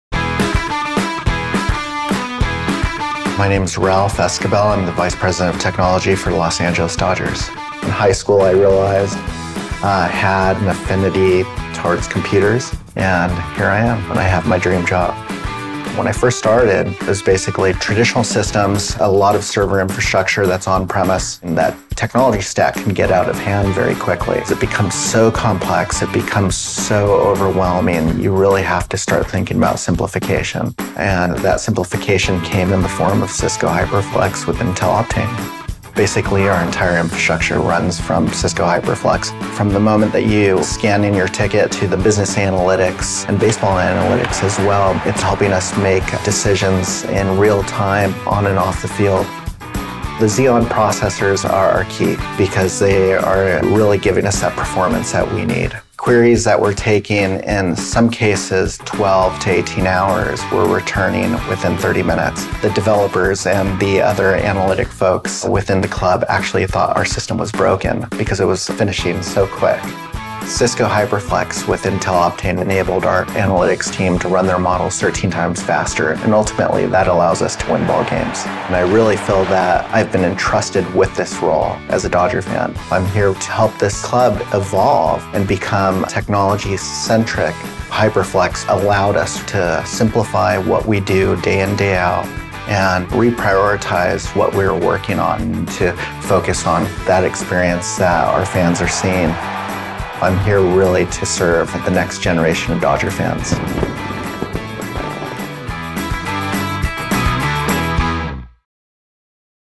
시스코 하이퍼플렉스 고객 사례 – LA 다저스
LA 다저스가 경기장 안팎의 고객 경험과 승률을 동시에 높이는 비결은 과연 무엇일까요?
시스코 하이퍼플렉스와 함께 미래의 야구를 현실로 만드는 LA 다저스의 성공사례를 만나보세요.